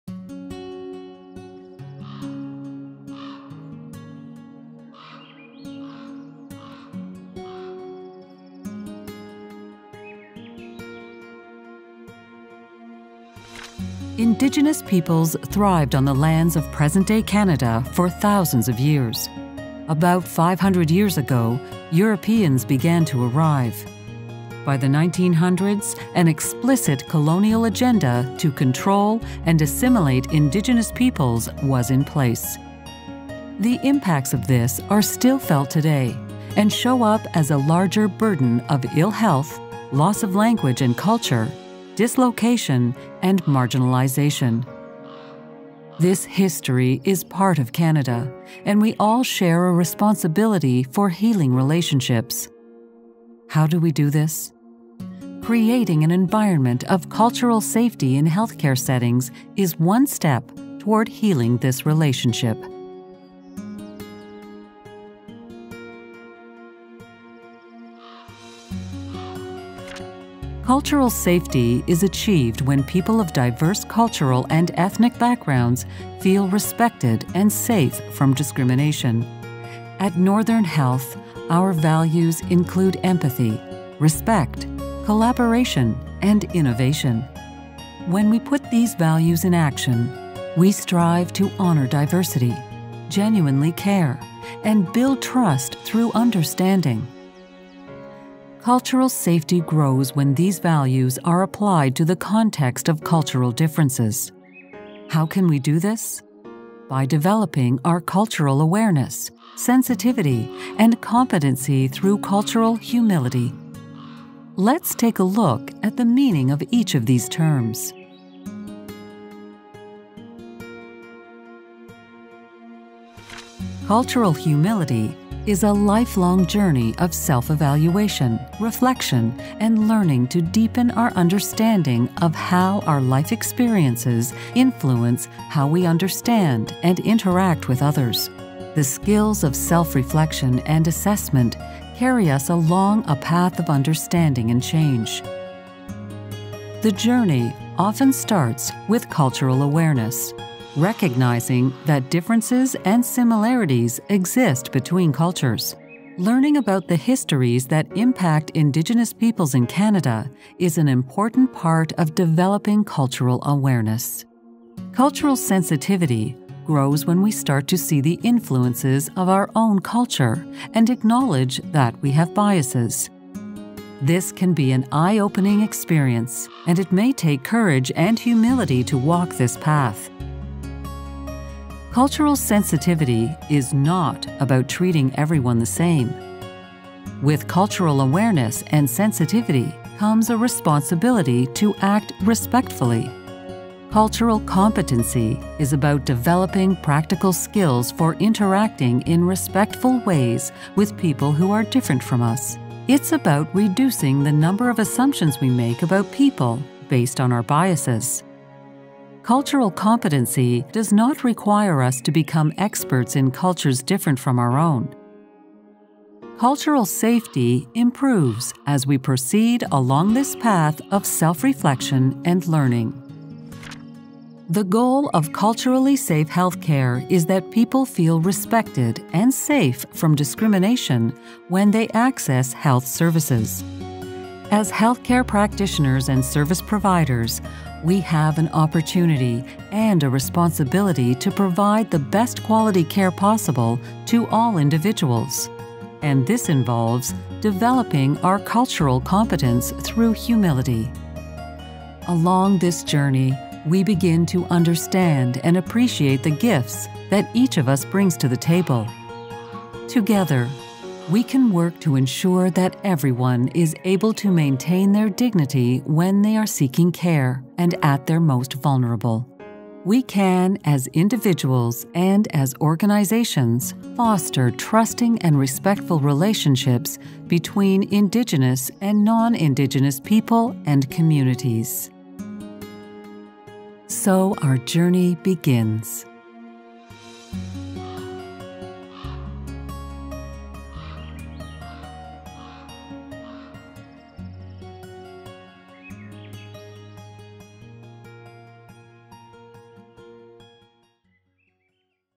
Cultural Safety: Respect and Dignity in Relationships
This 5 minute animated video introduces cultural safety and related concepts in an easily understandable way. It invites health care providers to participate in making the health system more culturally safe for Indigenous people and families. The video was produced by the Indigenous Health (formerly Aboriginal Health) team of Northern Health.